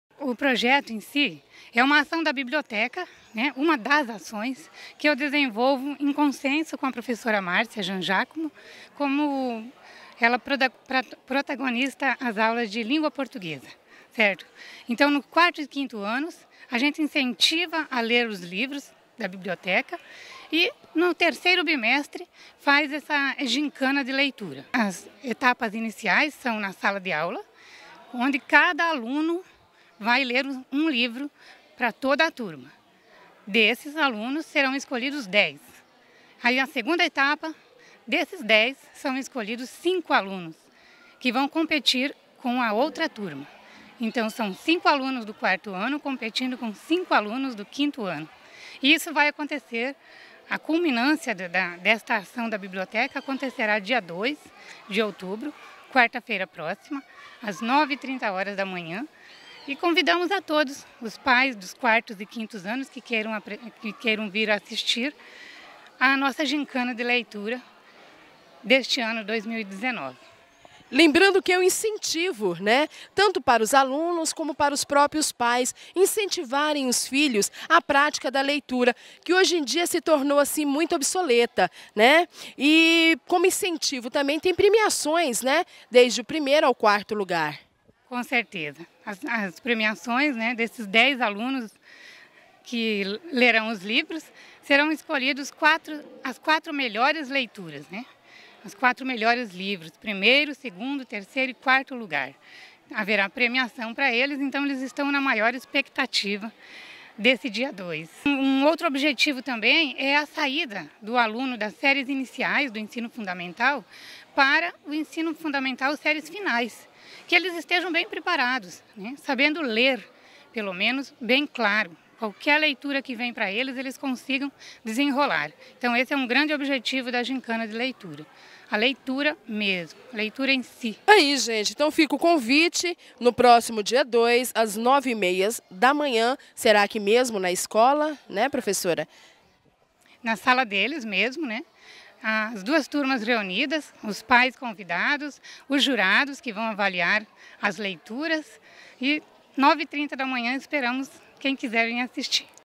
PROJETO "GINCANA DE LEITURA" SERÁ REALIZADO NA EMEB ANFILÓFIO DE SOUZA CAMPOS
RecordTV Canal 07